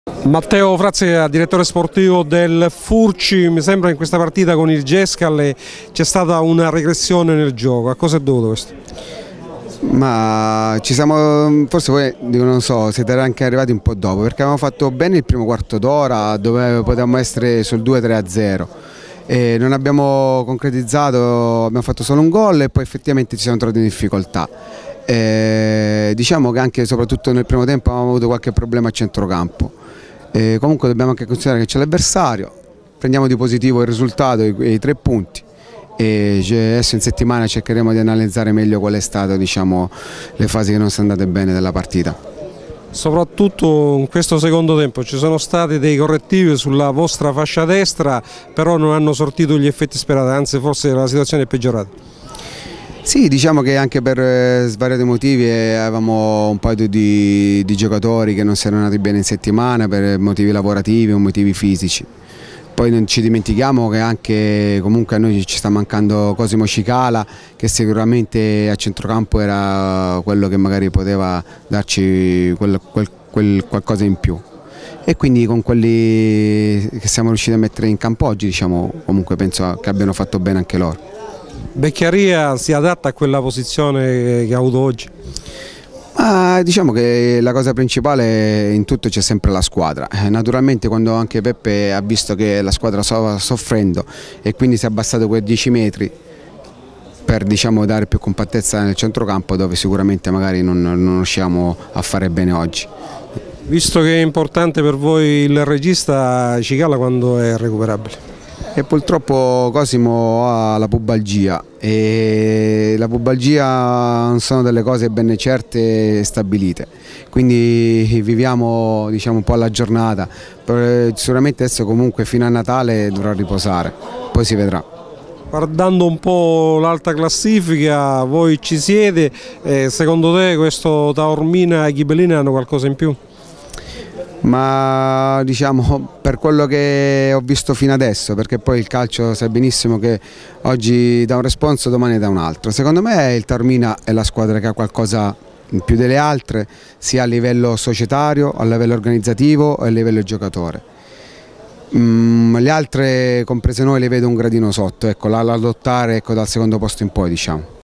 Intervista a Matteo Frazzica (Furci)
Intervista a Matteo Frazzica dopo la partita Furci-Gescal finita 1-0